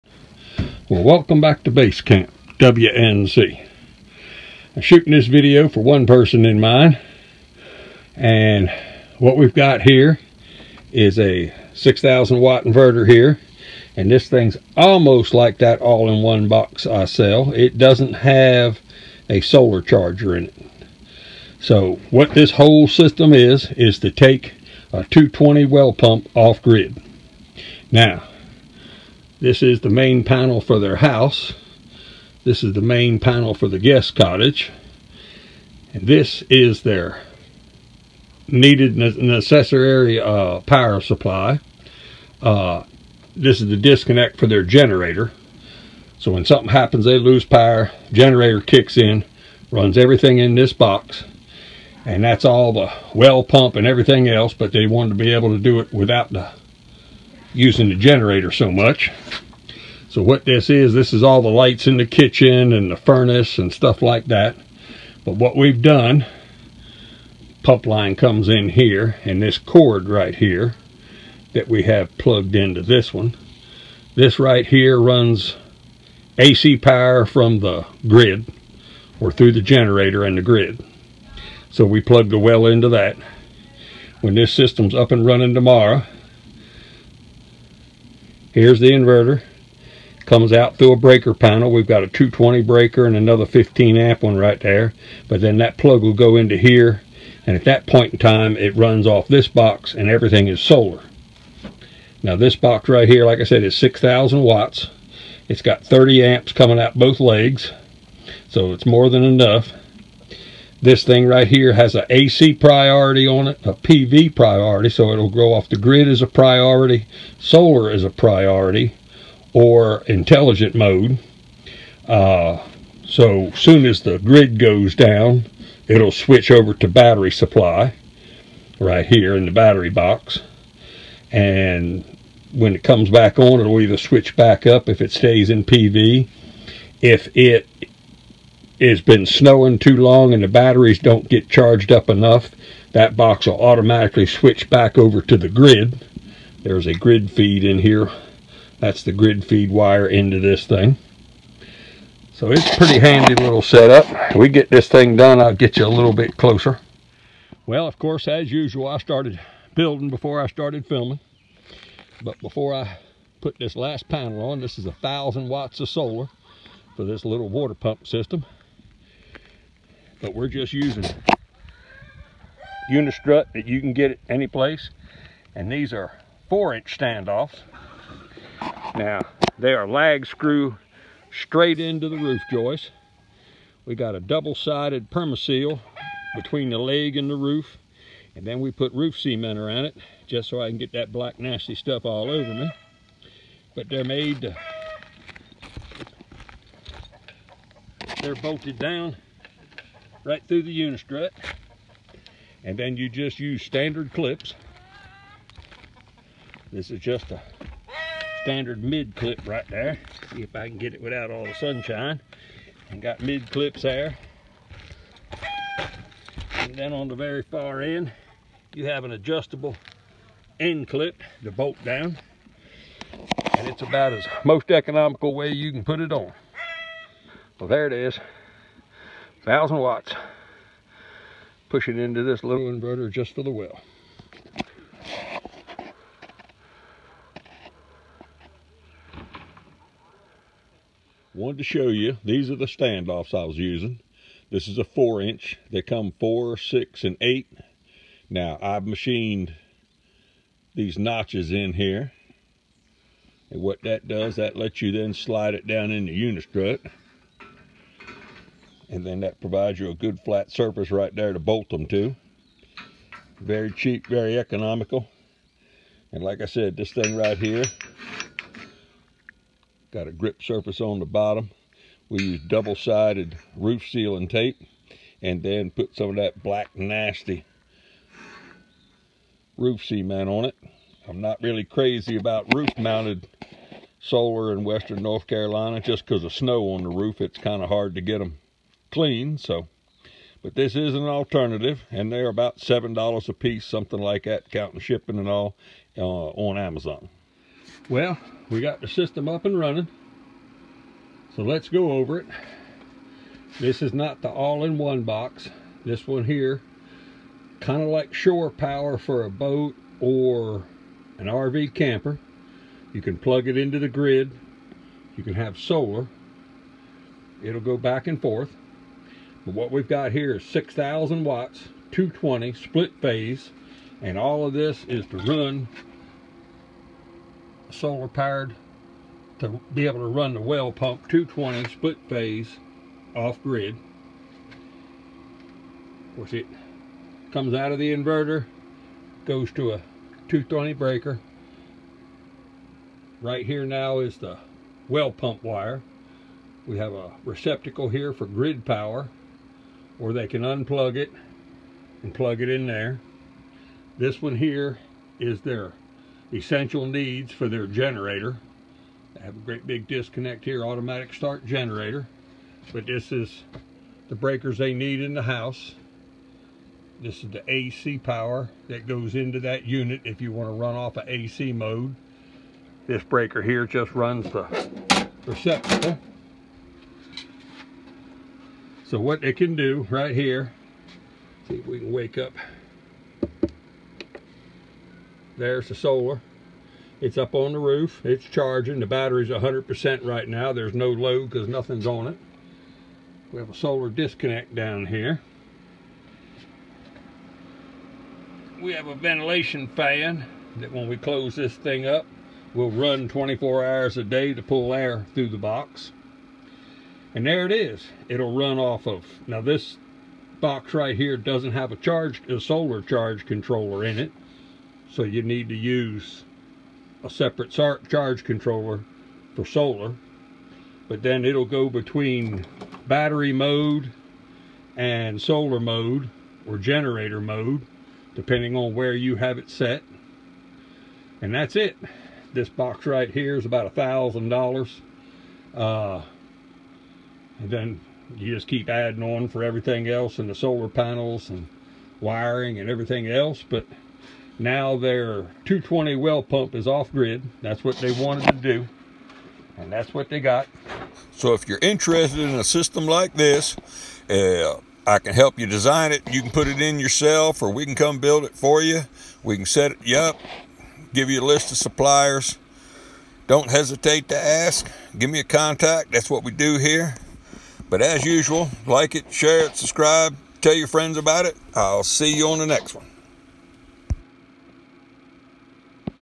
Taking A Deep Well Pump Off Grid With A Stand By Inverter
Here we are using a shore power 220 split phase inverter to take a deep well pump off grid
This method is about half price than the all in one box inverter price